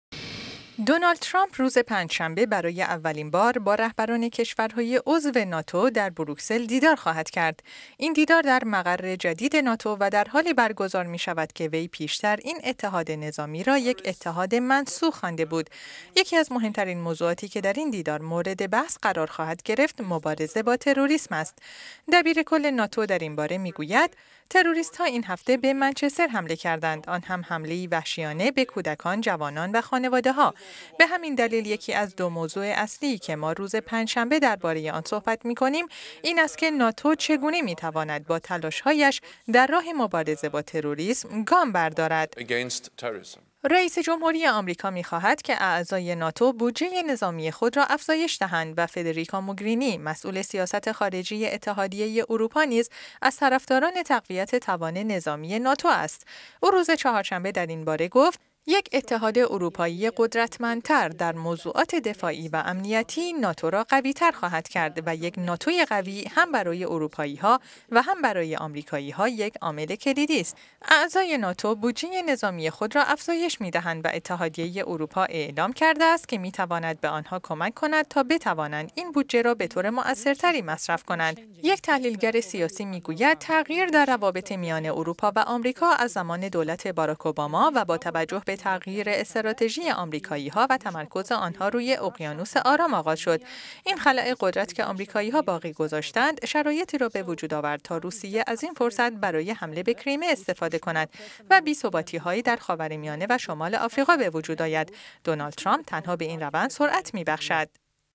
دیدار دونالد ترامپ با رهبران کشورهای عضو ناتو در بروکسل
دونالد ترامپ روز پنجشنبه برای اولین بار با رهبران کشورهای عضو ناتو در بروکسل دیدار خواهد کرد. این دیدار در مقر جدید ناتو و در حالی برگزار می شود که وی پیش تر این اتحاد نظامی را، یک اتحاد «منسوخ» خوانده بود.

یکی از مهمترین موضوعاتی که در این دیدار مورد بحث قرار خواهد گرفت، مبارزه با تروریسم است.

ینس استولتنبرگ، دبیر کل ناتو در این باره می گوید: «تروریستها این هفته به منچستر حمله  کردند آنهم حمله ای وحشیانه به کودکان، جوانان و خانواده ها. به همین دلیل یکی از دو موضوع اصلی که ما روز پنجشنبه دربار…